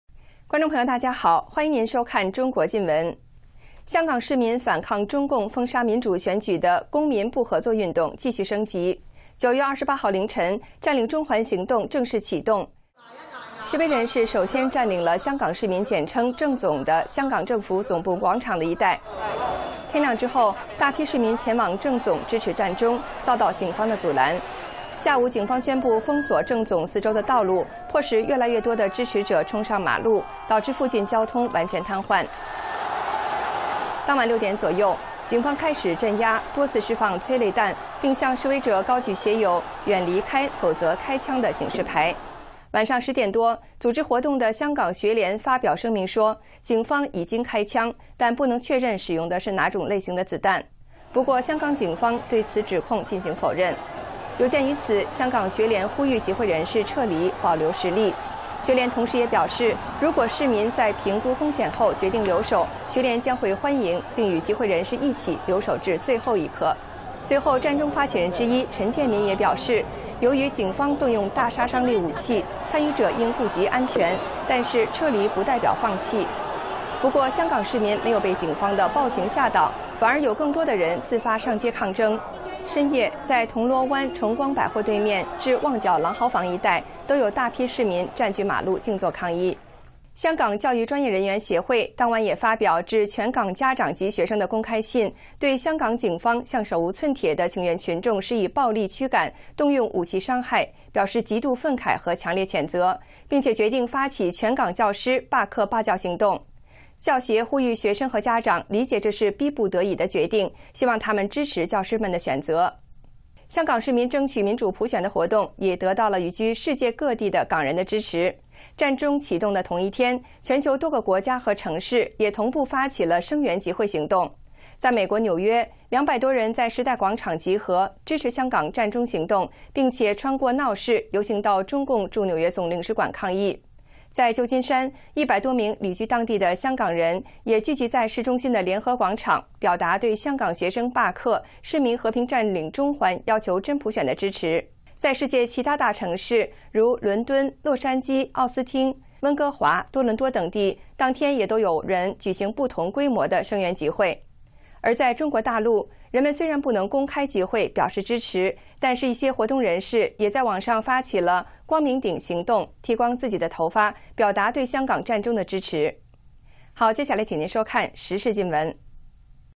Hong Kong Civil Central Occupy Launched. The Police Start Shooting
The Hong Kong civil disobedience campaign against the
Chinese Communist Party (CCP) force-out on democratic
election continues to escalate. On the morning of Sept. 28,
Central Occupy officially started.

The protesters first occupied a public square attached to the
government's headquarters.

After dawn, large numbers of people going to the square to
support Central Occupy were blocked by the police.

In the afternoon, the police announced they would seal off
roads around the square.

By forcing more and more supporters onto the road it would
bring the traffic to a standstill.

Later the police started repression by shooting several tear
gas canisters into the crowds.

They shouted warnings "Disperse Or We Fire" to the
protesters.

(HKFS) appealed to the rally for people to leave and preserve
strength because the police were shooting possibly using
live bullets.

HKFS also said that people are welcome to stay after the
risk assessment.

They will stay with people until the last minute.

that participants' safety was important, because the police
were using high killing power weapons. However, leaving
doesn't mean abandoning.

However, Hong Kong police denied the shooting allegation.

Hong Kong people were not intimidated by
the police brutality.

More people spontaneously took to the street to fight.

According to Hong Kong Apple Daily report, many people
protested on the road by sitting-in opposite the Sogo
department store in Causeway Bay and at Mongkok area.

Hong Kong Professional Teachers' Union Issues Open
Letter to appeal Teachers Strike

Hong Kong Professional Teachers' Union (HKPTU) expressed
extreme indignation and strong condemnation with the Hong
Kong police's violent clear out and using weapons on unarmed
petitioners in an open letter to Hong Kong parents and students.

They decided to launch a teachers strike in Hong Kong.

KHPTU hopes students and parents can understand the appeal.

They hope students and parents understand the forced decision
and support the teachers.

Global Solidarity For Hong Kong's Struggle For Democracy.

Hong Kong democratic activities for Universal suffrage also
got support from Hong Kong people around the world.

On the same day of the Central Occupy, solidarity rallies
were simultaneously launched in many countries and cities.

In New York, two hundred people gathered in Times Square
to support Hong Kong Central Occupy.

They protested at the Chinese Communist Party (CCP)
Consulate General in New York in downtown.

In San Francisco, more than 100 local Hong Kong people also
gathered in Union Square.

They were supporting the Hong universal suffrage
request by peaceful Central Occupy.

There are different sizes of solidarity rallies in other big cities
such as London, Los Angeles, Austin, Vancouver, Toronto etc.

People can’t have public meetings in the mainland;

So some activists also launched Bright Peak Campaign
shaving their hair to express support to Central Occupy.